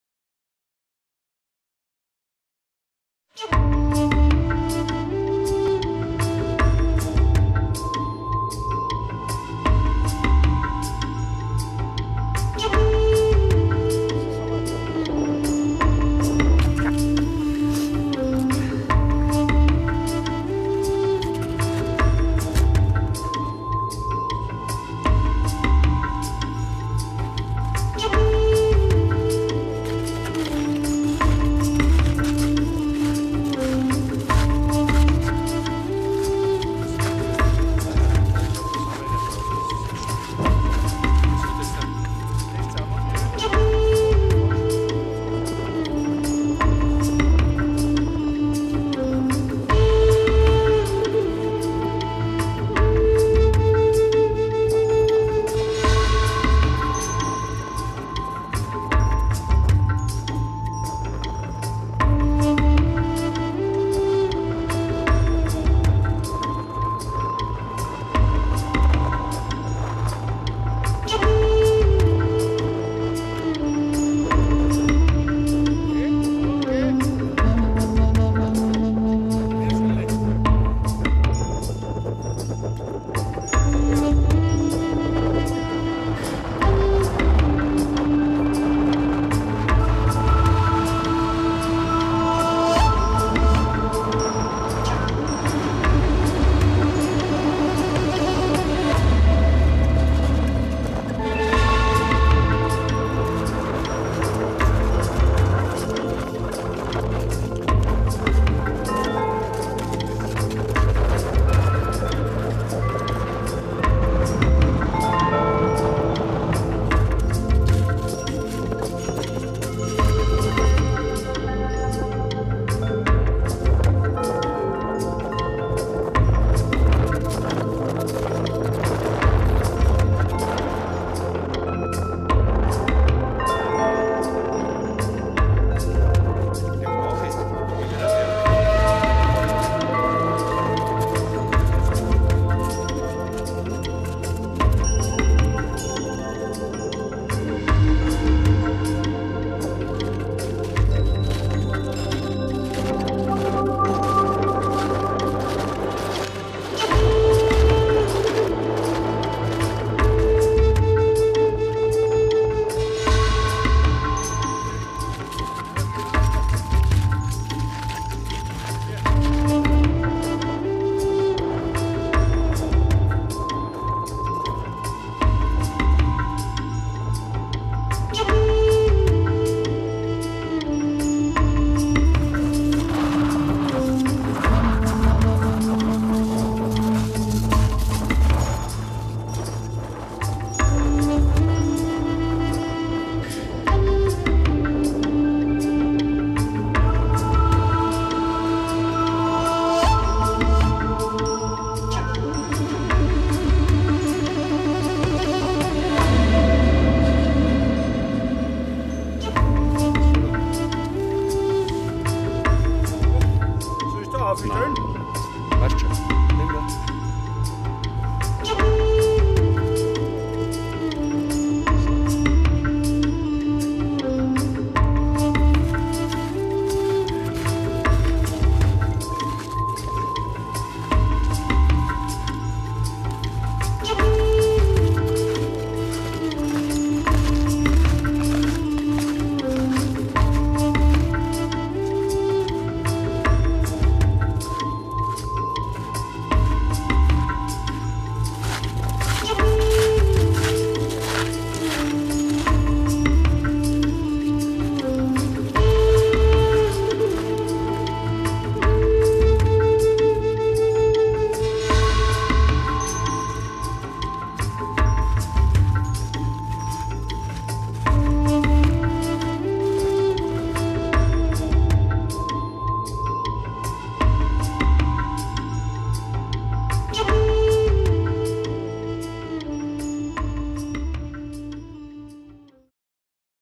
Bergtour auf den Speikkogel - EWG Oldies on Tour
Schneeschuhwanderung vom Salzstiegl auf den Speikkogel
Ein Film von Wolfgang Moik